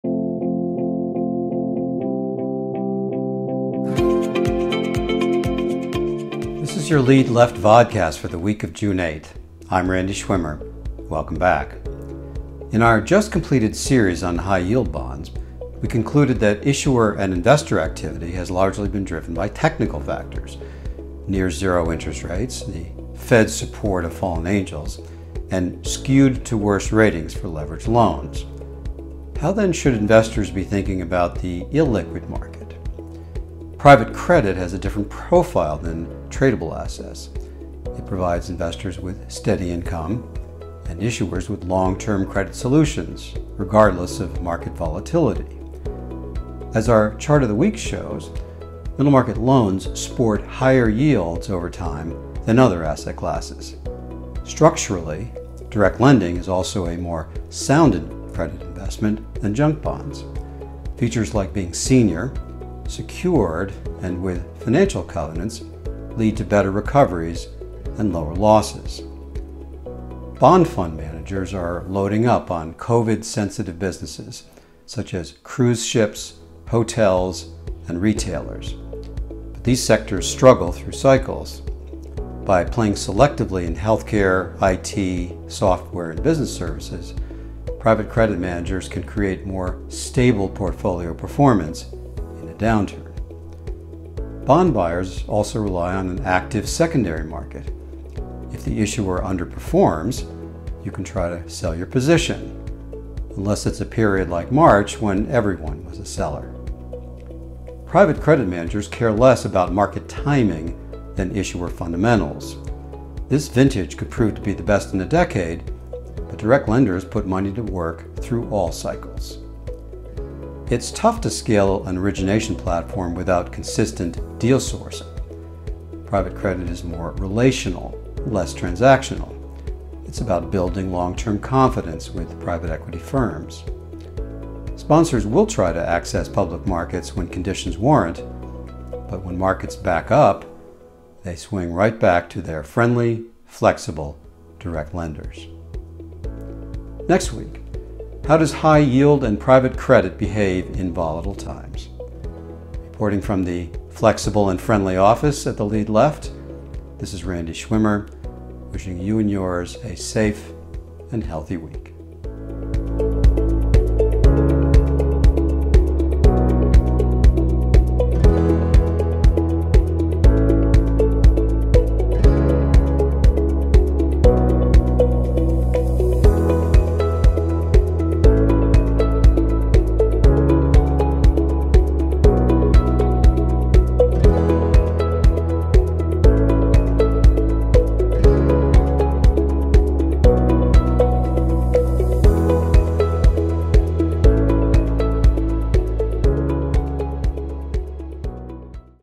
High-Yield and Private Credit
In our just-completed series on high-yield bonds, we concluded that issuer and investor activity has largely been driven by technical factors: near-zero interest rates, the Fed’s support of fallen angels, and skewed-to-worse ratings for leveraged loans.

How then should investors be thinking about the illiquid market?

Private credit has a different profile than tradable assets. It provides investors with steady income (and issuers with long-term credit solutions), regardless of market volatility.

As our Chart of the Week shows, middle market loans sport higher yields over time than other asset classes...